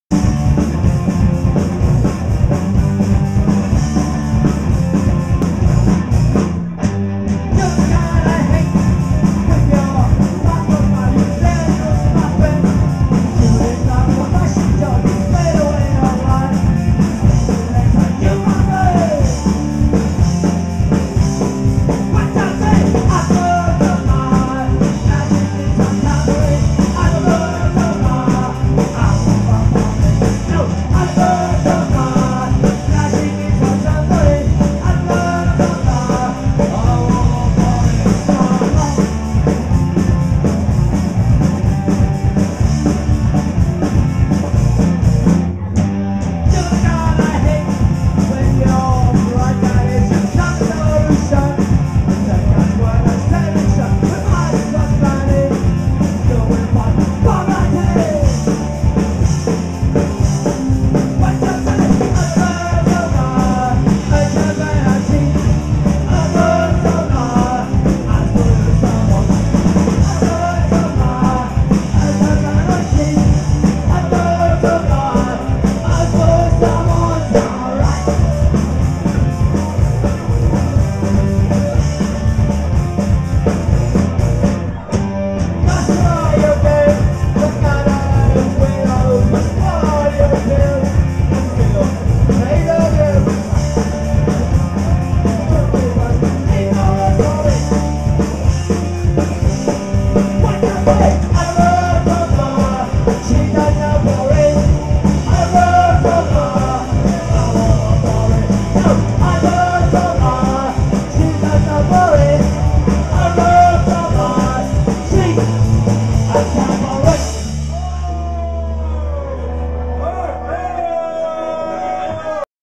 Box of brats (2) - Live du 31/01/15
Festival Le cri du ralliement avec Atomics Rotors (Psychobilly Garage Surf - Narbonne) + Lazy Doll Factice (Punk Rock - Vaucluse) + 8more8 (Punk Hardcore - Nîmes) + Box Of Brats (Punk Rock - Forcalquier) à le "86" @ Remoulins (30)